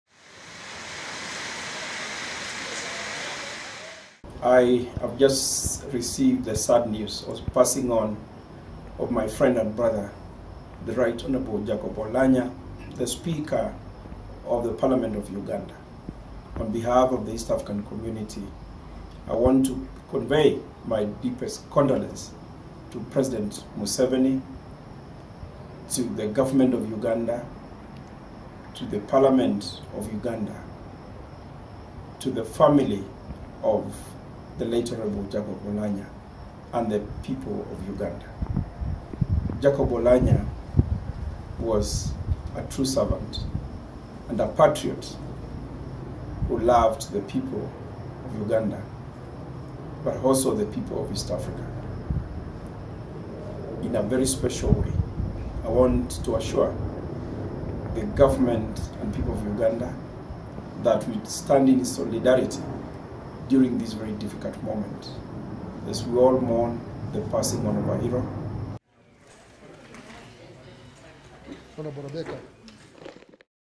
EAC Secretary General eulogises Jacob Oulanyah
The Secretary-General of the East African Community Dr Peter Mathuki has sent messages of condolence to Uganda on the death of the Speaker of Parliament Jacob Oulanyah.

Oulanyah has been unwell for some time and was hospitalised in Seattle, USA.